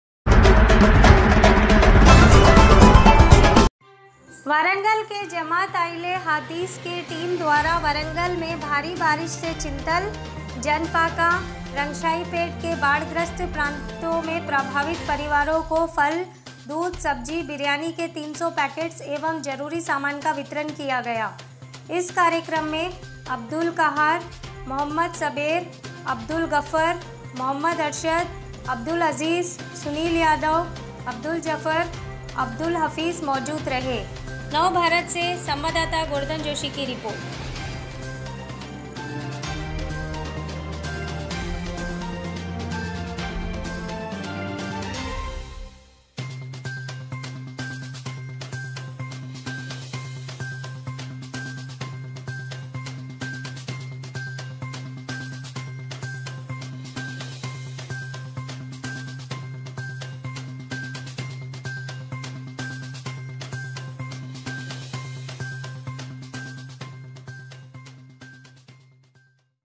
वरंगल जमात आईले हदीस के नेतृत्व मे बाढग्रस्त परिवारो भोजन फल अन्य सामान वितरण किया गया
Goverdhan Joshi / 9 Bharat Samachar